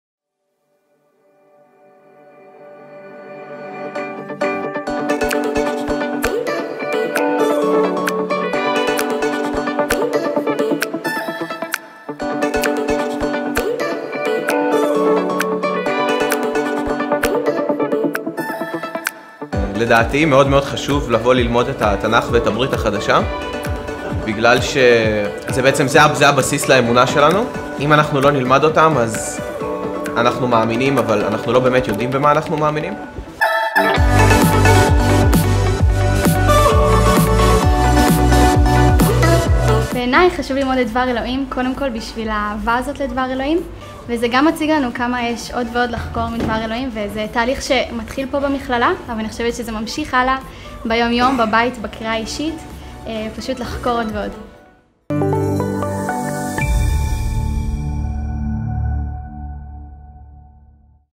ONE FOR ISRAEL'S Bible College - Israeli students share why it's so important
ONE FOR ISRAEL'S Bible College was established for the purpose of training the rapidly growing community of followers of Jesus in Israel. We are, in fact, the only accredited Hebrew speaking seminary in the world! Jewish and Arab believers come from all over Israel to study the Bible, theology, counseling, and practical ministry.